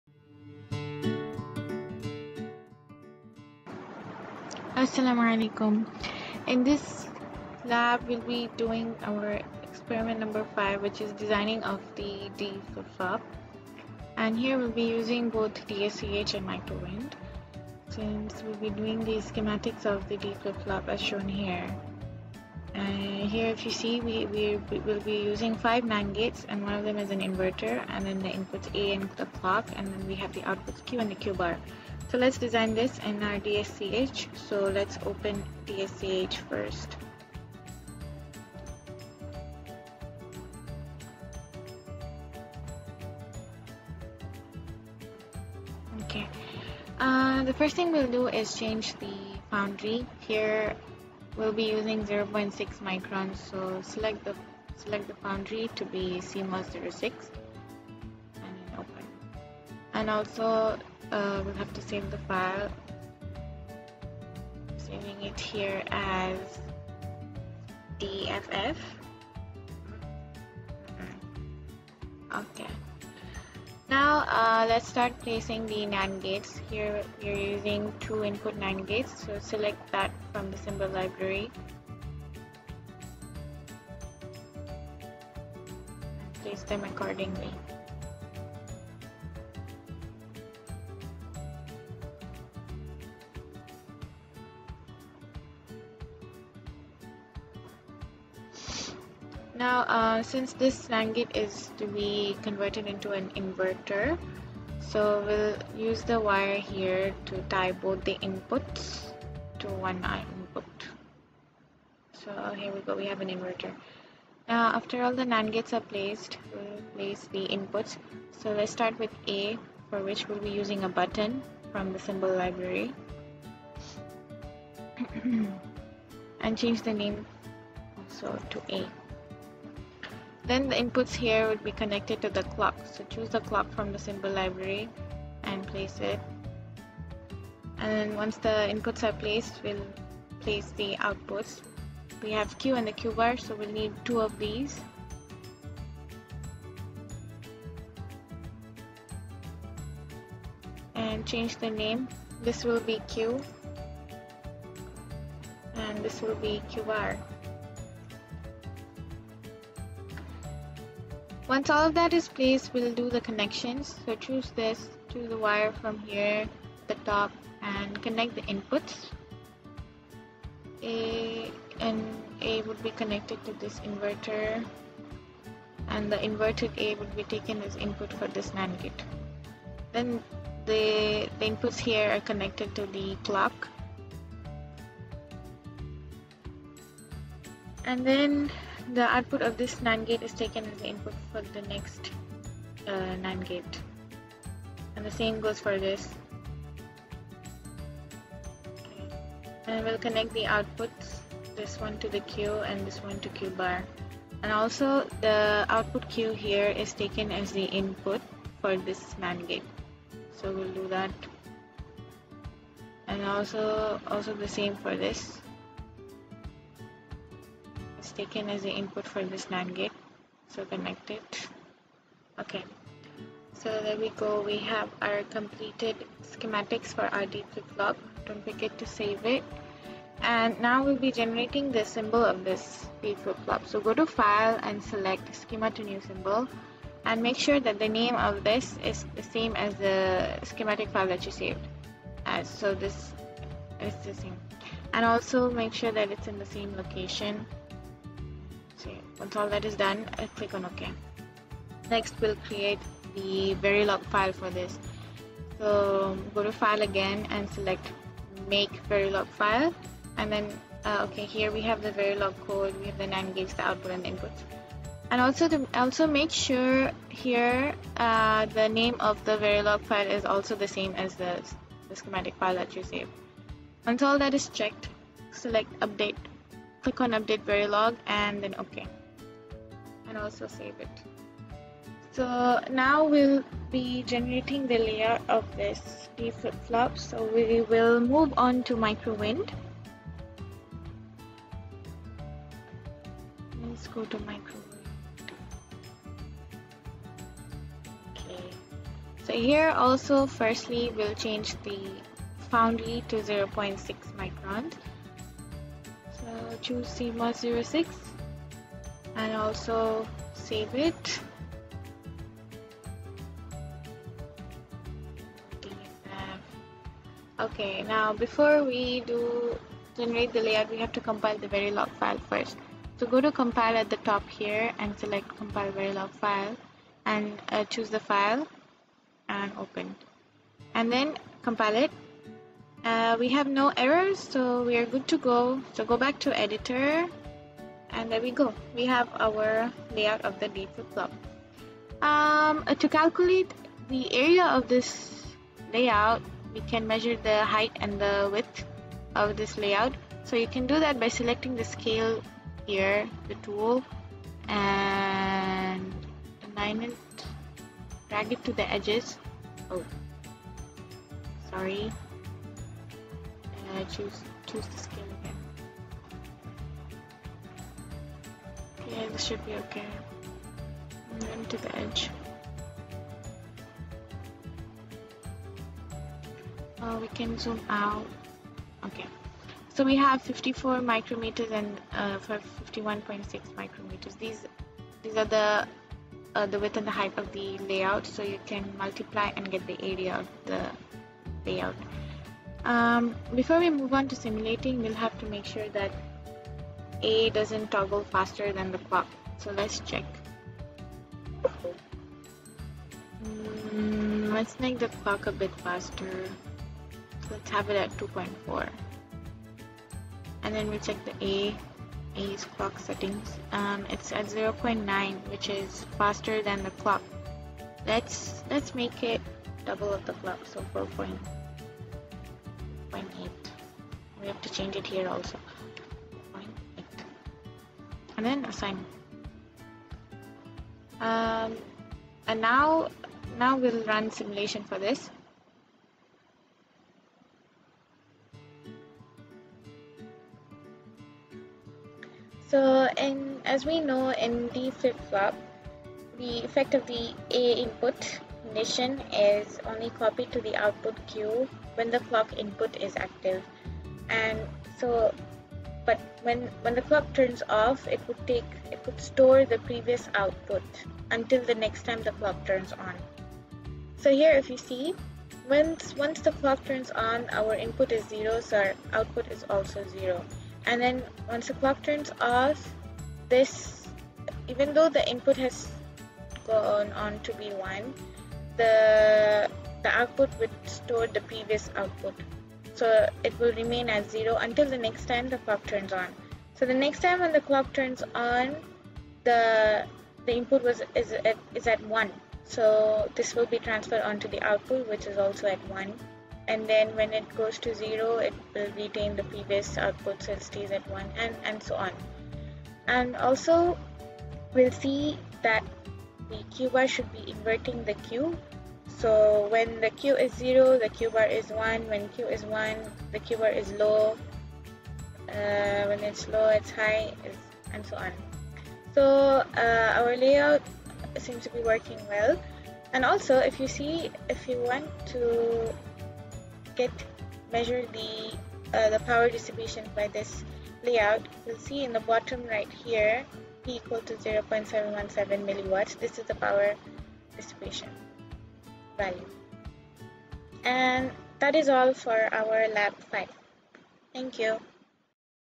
Lab 5: Design of a D-Flip Flop
Describes the design of a D Flip Flop using Microwind